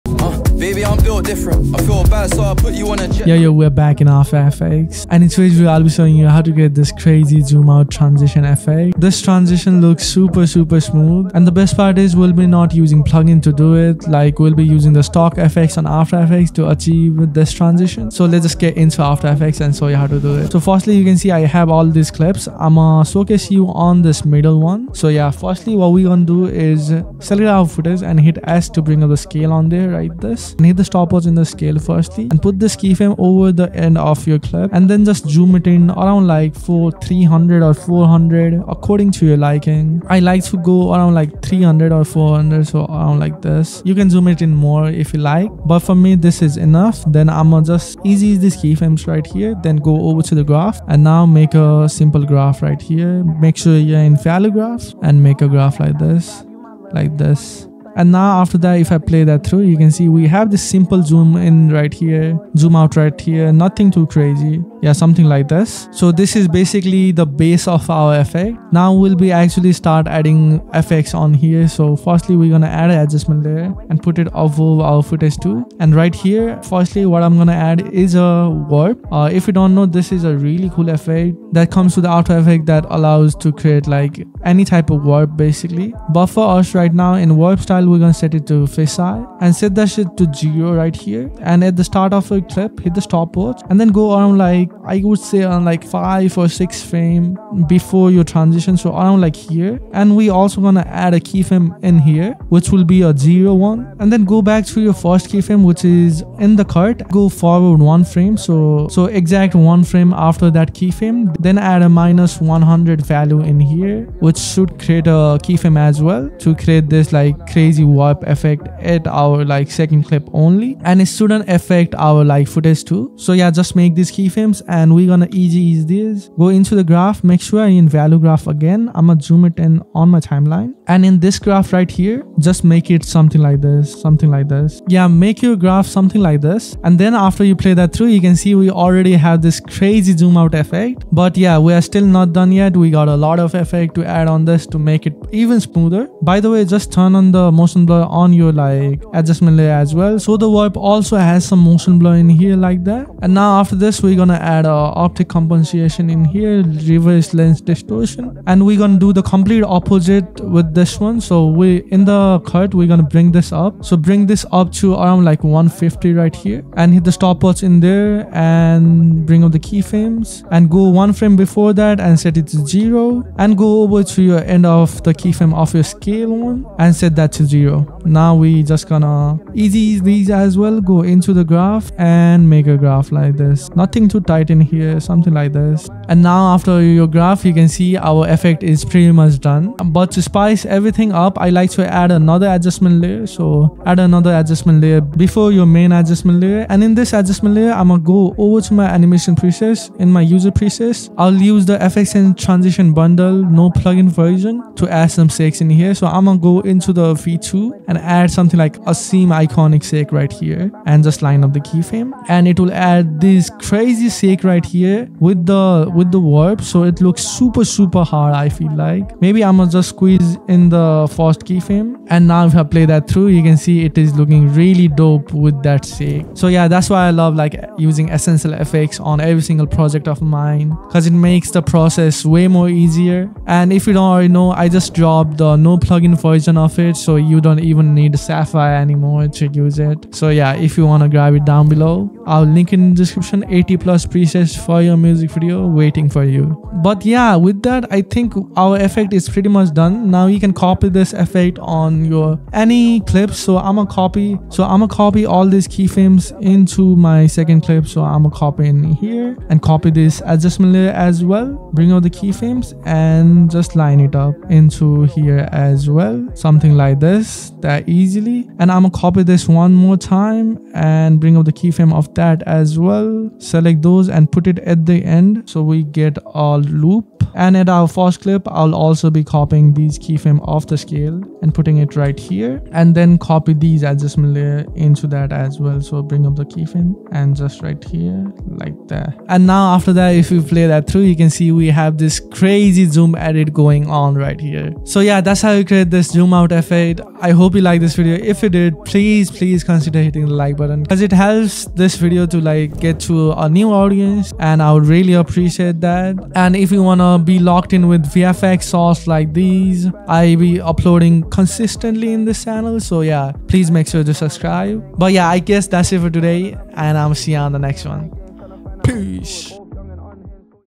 SMOOTH ZOOM OUT Transition in After Effects *NO PLUGIN*
In this tutorial you will learn how to make SUPER SMOOTH MUSIC VIDEO TRANSITIONS (After Effects + Plugin)

Timestamps:
00:00 - Overview
00:20 - SCALE Animation
01:08 - Warp & Optics
03:18 - SHAKE
04:07 - Looping the effect
04:48 - Outro